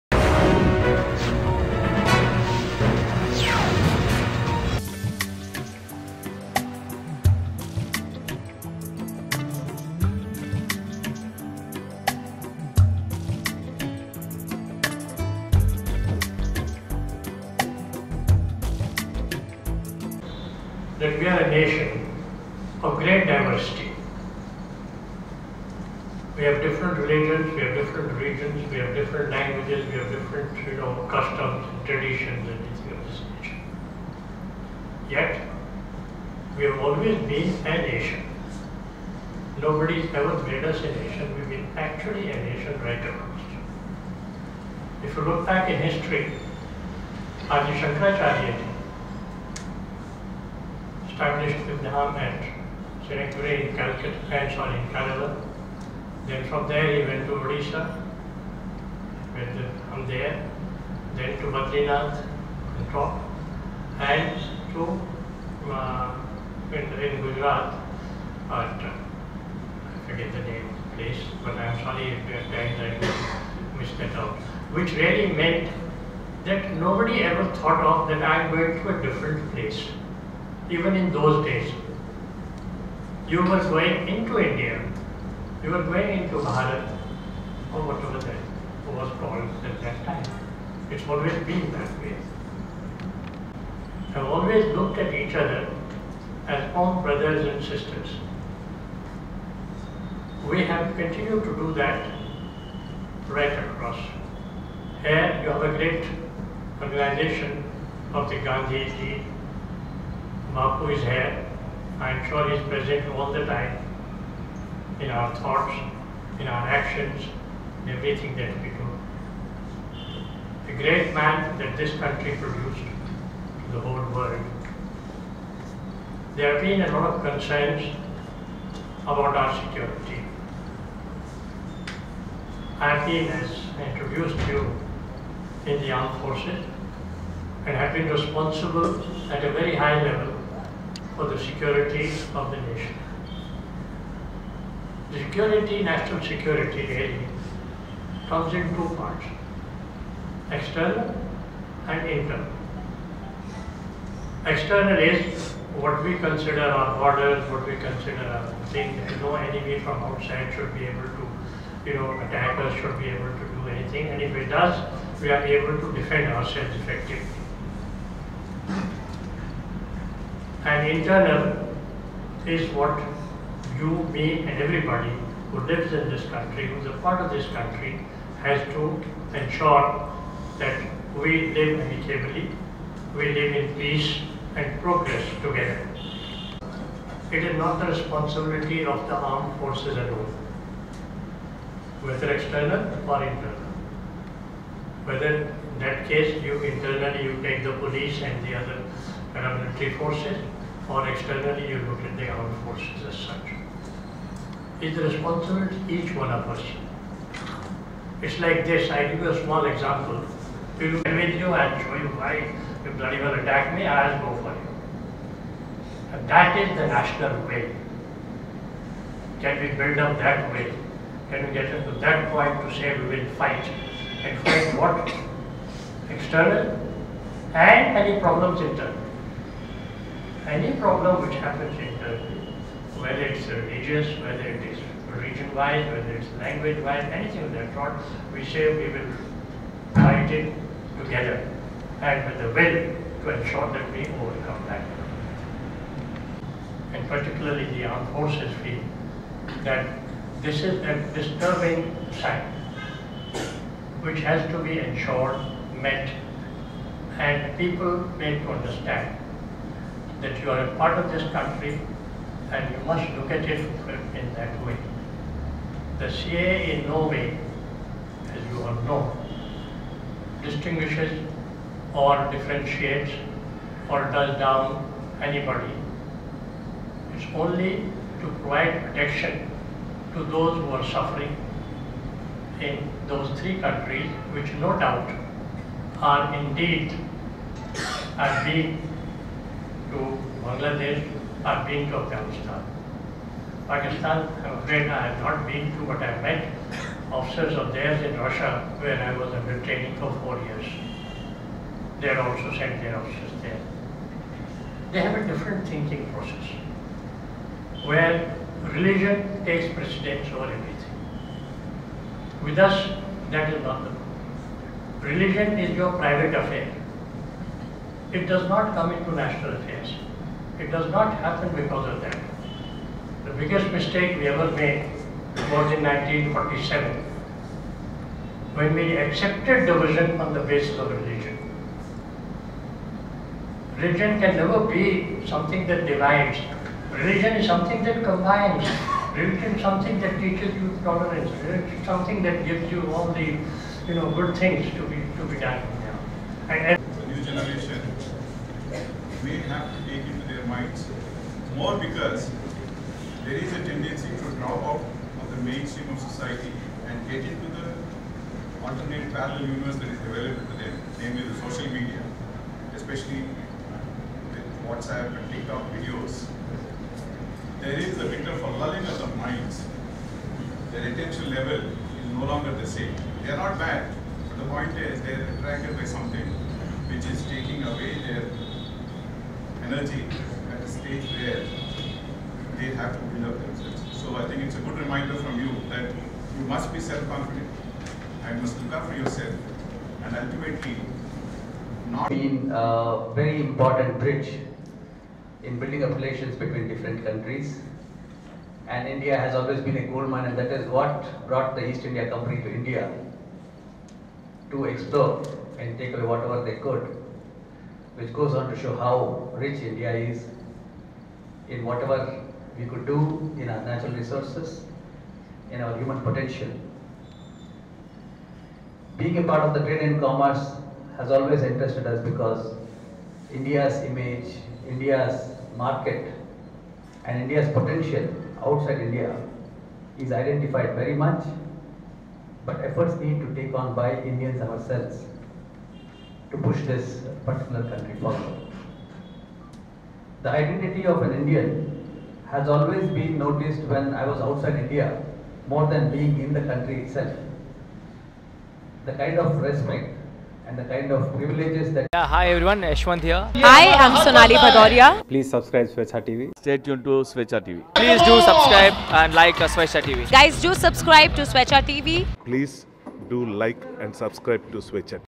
Retired Deputy Chief of Army Lieutenant General N S Malik Speech at Lankapalli Bullayya College
లంకపల్లి బుల్లయ్య కళాశాల బ్లాక్ 5 సమావేశ మందిరంలో విశ్రాంత డిప్యూటీ చీఫ్ ఆఫ్ ఆర్మీ లెఫ్టినెంట్ జనరల్ ఎన్ ఎస్ మాలిక్ గారు, ఇండియన్ ఎకనమిక్ ట్రేడ్ ఆర్గనైజేషన్ అధ్యక్షులు డా ఆసిఫ్ ఇక్బాల్ గారు కళాశాల విద్యార్థులను, అధ్యాపకులను ఉద్దేశించి  "అంతర్గత భద్రత- అంతర్జాతీయం గా భారత్ ఎదుర్కొంటున్న సవాళ్లు" అనే అంశంపై ప్రసంగించారు.